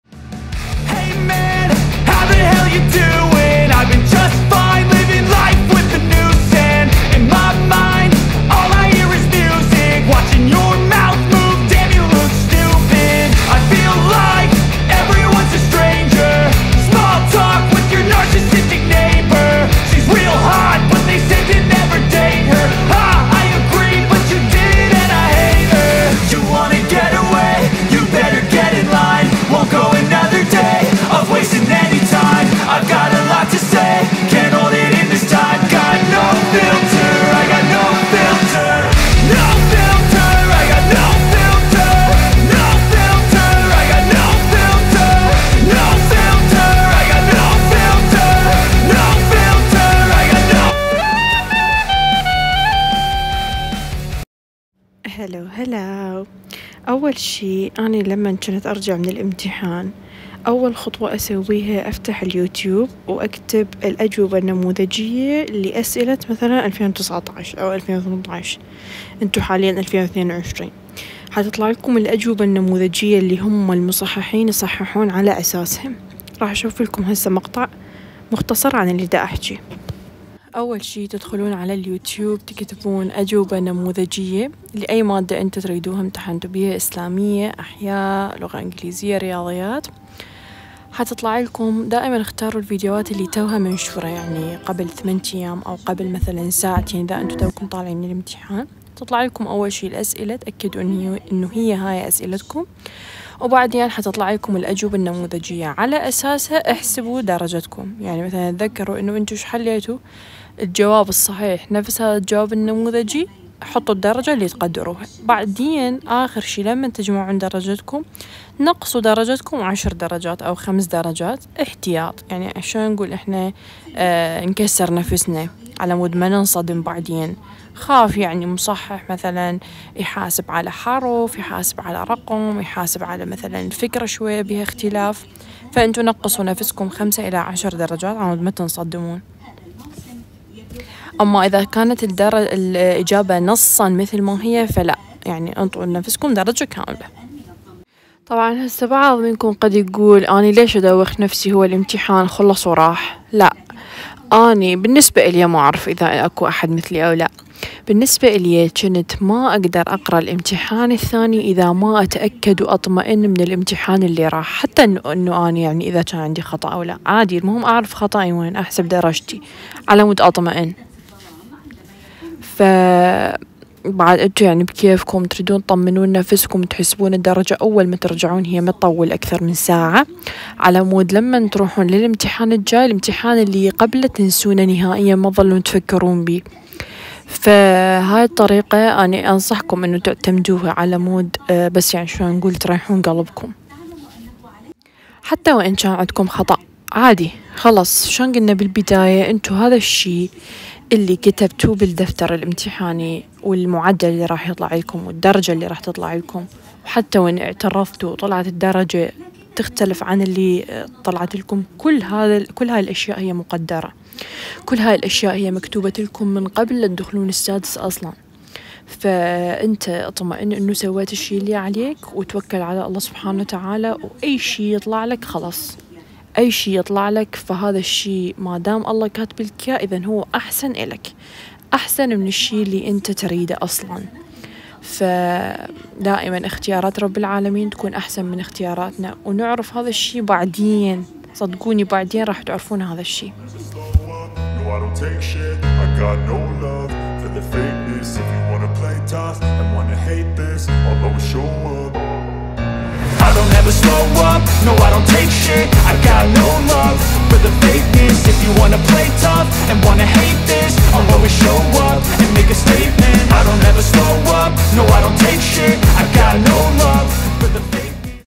شلون تحسب درجتك بعد الامتحان الوزاري بشكل صحيح | لطلاب السادس اعدادي مهم جدا | 2022
اعتذر طبعا الفديو الي نشرته قبل ساعتين انمسح من عده الصوت ماعرف ليش فحذفته ورجعت حملته 😭😭😭😭😭💔

الحمد لله
سبحان الله
الله واكبر